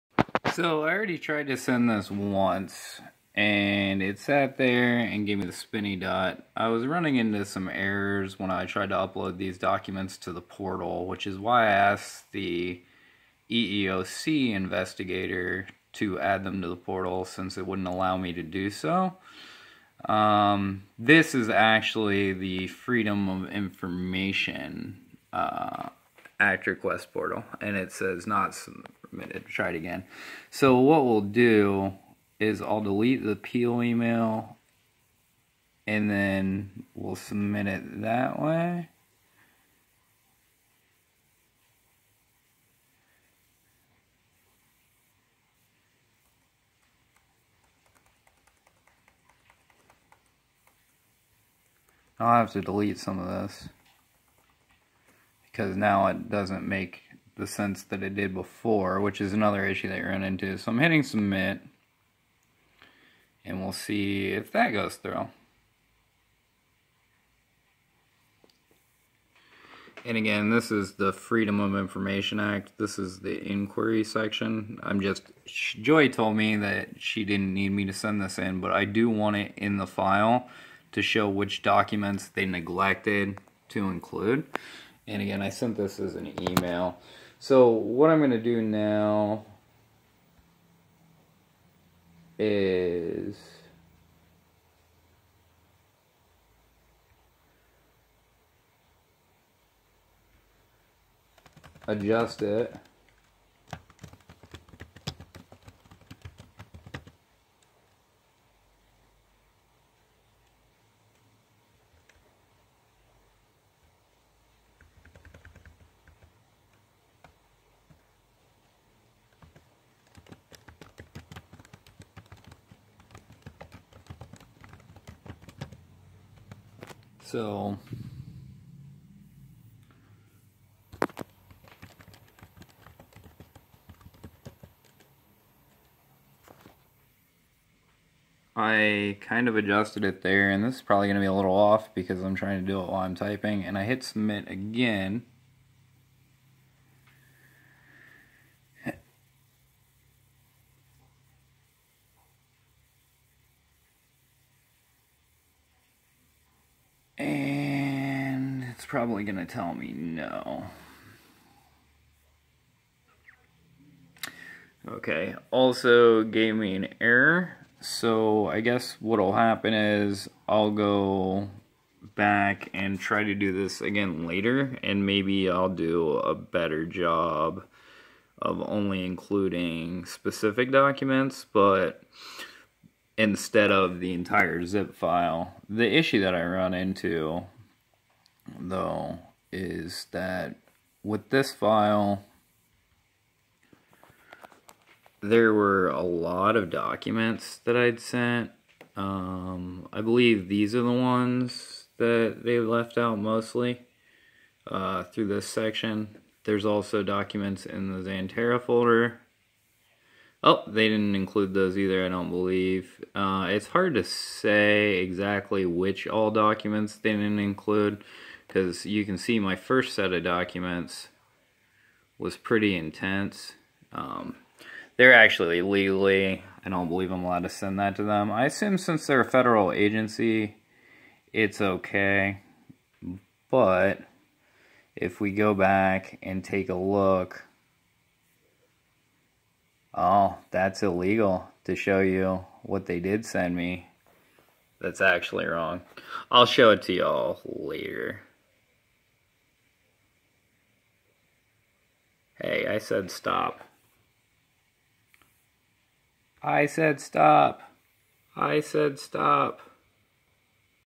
EEOC Foia attempt to send missing documents. Error.
00:00 Received 04/24/2021 19:45
00:05 Due 05/24/2021 Priority Normal
00:10 Modified 07/19/2021 16:44
00:15 I received an Email from the FOIA department advising to follow up with Dilip Gokhale in regard to the documents I sent them that Brian Shelton Neglected to include in the investigation that I had sent him on October 15th, 2019 in the Zip file that would not upload to the portal from the website. As there were some issues in his handling and notations I thought it would be best to forward to you to review. This was just my initial review of the documents as the FOIA advised to send you the email immediately due to the deadlines involved.

Robert Lust

I was advised to send the previous email in that format however I did have these additional notes regarding the file based on a preliminary read.

I was reviewing your notes and saw some discrepancies. First of all I did not say a gay man was the same as a woman. The situation I was describing to you was a Gay man and a transexual who Identifies as a Male but was a woman so I may have confused you that is not how I feel about the situation and makes it appear that I am biased in some way against the gay community which I did not appreciate. I was trying to describe the situations of other instances that similar situations may have occurred per the investigators request.

I also was not provided a list of attorneys in the proper Jurisdiction was my concern and the way you worded it appears I would not have accepted that.

The other issue is that the Employer was allowed to file two extensions for their response and I was not provided that information. These types of issues make it very difficult for someone in this situation especially when no investigation was done properly and the employer has all the information to prepare a defense.. Mediation was closed on 01/06/2021 and they filed two extensions to prepare a position statement the last being until April 19,2021.

The letter I received was dated on 3/25/2021 from your office which was closed before ever even being provided a response from the company.

As the Company had requested two extensions to the deadline was the Position statement ever received and if so why not.

Robert Lust
I received the FOIA request and many of the emails were not included, investigated, or considered in the investigation of my charge. The most relevant of which was the Zip file containing word documents, texts, and other documentation that was relevant to the case of discrimination. I reported to HR that an employee was spreading slanderous information and false accusations as well as provided HR with documentation this was the case and witnesses to verify, but because I was a male they not only refused to review the information properly or follow up with the witnesses I provided they neglected to include that information in their files or forward it for the proper people to review. Instead they created a hostile work environment for me and made it impossible for me to stay at the location for fear of retaliation. I also provided documentation to you in the form of a FOIA request that the person had been saying that I sexually assaulted them and that what they were saying was sexual assault was in fact not. I was sexually harassed by that person at work and avoided them due to discomfort along with the fact she said they may have knowingly given me an STD and none of that was considered.

01:00 The Female who made the false accusation was moved to another better location prior to me being suspended and put on house arrest. The company did not take her written statement until after I filed the complaint against her and it was not investigated at all in context to the actual facts. They also withheld evidence that contradicted her statements including a possible security report for which I provided them the date and security officer who was involved on our last sexual encounter when she drug me to her room while I was inebriated and after I had refused her multiple time (there was a witness to this) and the security officer came to the room because we were being too loud after curfew but it was not do to a negative encounter and she was the one who spoke directly to the officer.

2:00 I also provided HR with the Date and Time as well as the person who rode with me to the clinic after this encounter because she had told me not to wear a condom and she needed a morning after pill, a STD test, and pregnancy test because she had sex with multiple people in the recent weeks and she requests that her sexual partners do not use condoms.

3:00 There are more details provided in the zip file and I can go into more specifics but the Lawyer I spoke to advised that I am overcomplicating this with details and it was a straightforward case of discriminations and hostile work environment.

Thanks,
Robert Lust